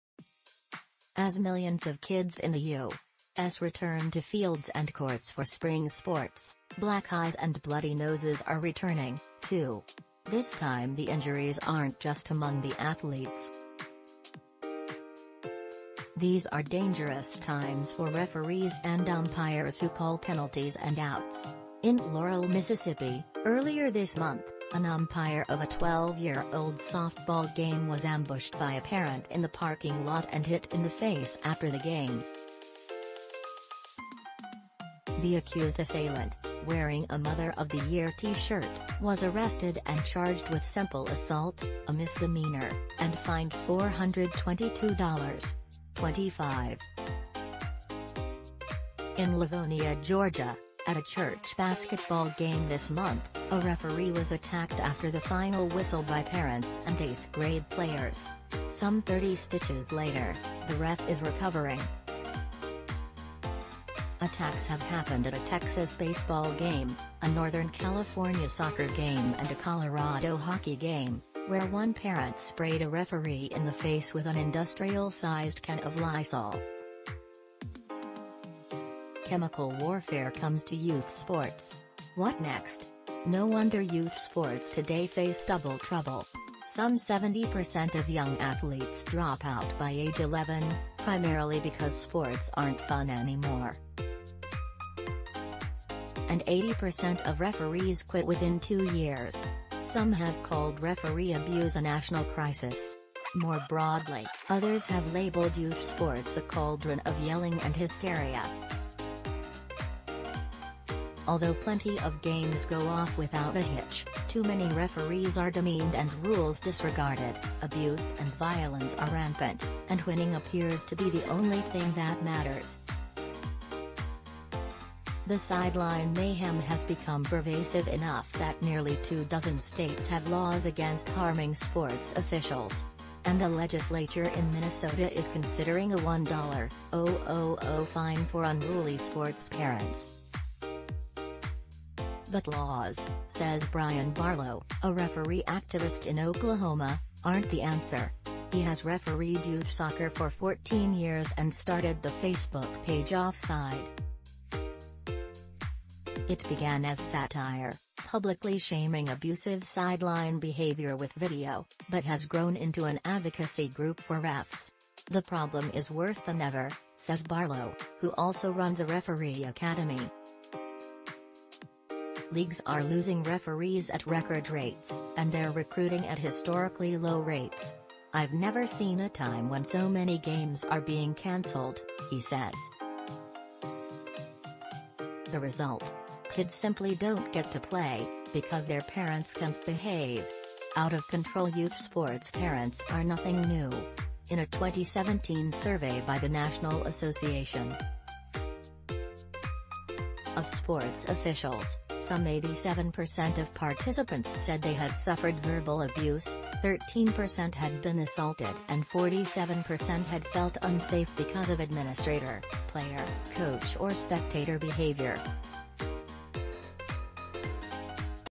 Op Ed Angry parents are ruining youth sports Here’s how to rein them in
As millions of kids in the U. S. return to fields and courts for spring sports, black eyes and bloody noses are returning, too. This time the injuries aren’t just among the athletes. These are dangerous times for referees and umpires who call penalties and outs. In Laurel, Miss., earlier this month, an umpire of a 12-year-olds’ softball game was ambushed by a parent in the parking lot and hit in the face after the game. The accused assailant — wearing a “Mother of the Year” T-shirt — was arrested and charged with simple assault, a misdemeanor, and fined $422. 25. In Livonia, Ga., at a church basketball game this month, a referee was attacked after the final whistle by parents and eighth-grade players. Some 30 stitches later, the ref is recovering. Attacks have happened at a Texas baseball game, a Northern California soccer game and a Colorado hockey game, where one parent sprayed a referee in the face with an industrial-sized can of Lysol. Chemical warfare comes to youth sports. What next? No wonder youth sports today face double trouble. Some 70% of young athletes drop out by age 11, primarily because sports aren’t fun anymore. And 80% of referees quit within two years. Some have called referee abuse “a national crisis. ” More broadly, others have labeled youth sports “a cauldron of yelling and hysteria. ”Although plenty of games go off without a hitch, too many referees are demeaned and rules disregarded, abuse and violence are rampant, and winning appears to be the only thing that matters. The sideline mayhem has become pervasive enough that nearly two dozen states have laws against harming sports officials. And the Legislature in Minnesota is considering a $1, 000 fine for unruly sports parents. But laws, says Brian Barlow, a referee activist in Oklahoma, aren’t the answer. He has refereed youth soccer for 14 years and started the Facebook page Offside. It began as satire — publicly shaming abusive sideline behavior with video — but has grown into an advocacy group for refs. The problem is worse than ever, says Barlow, who also runs a referee academy. Leagues are losing referees at record rates, and they’re recruiting at historically low rates. “I’ve never seen a time when so many games are being canceled, ” he says. The result? Kids simply don’t get to play — because their parents can’t behave. Out-of-control youth sports parents are nothing new. In a 2017 survey by the National Assn. of Sports Officials, some 87% of participants said they had suffered verbal abuse, 13% had been assaulted and 47% had felt unsafe because of administrator, player, coach or spectator behavior. And when it’s all caught on cellphone video, it’s increasingly hard to brush aside these incidents as isolated events. There’s some reason to believe that this most recent wave of violent behavior — also seen on planes, at grocery stores, at the Oscars — has been fueled by the pandemic.